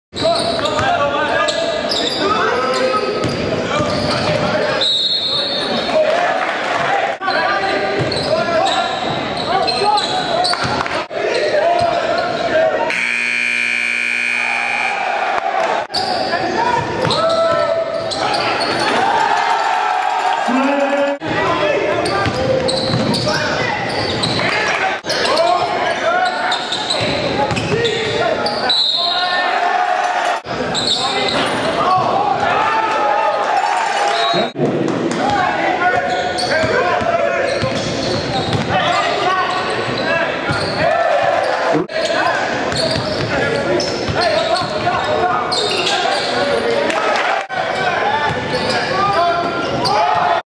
Pflueger highlights vs Palisades 12-21-11
Devon (#11, Jr.) and Rex (#1, Fr.) highlights in JSerra vs Palisades HS basketball 12-21-11